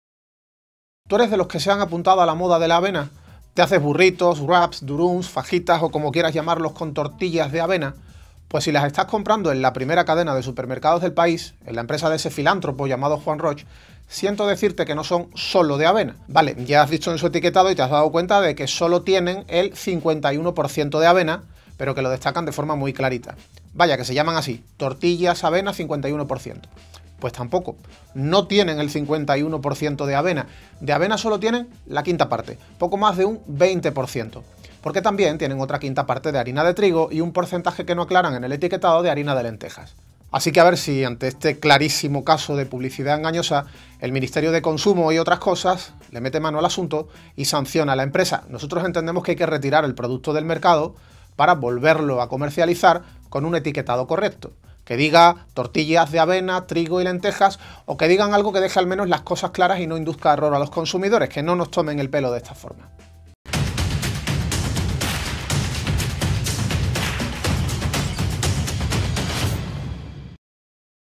Publicidad engañosa de Mercadona: sus "tortillas de avena 51%" no tienen el 51% de avena
FACUA pide la retirada de las "Tortillas de Avena 51%" de Mercadona: en realidad, este cereal solo supone la quinta parte de su composición.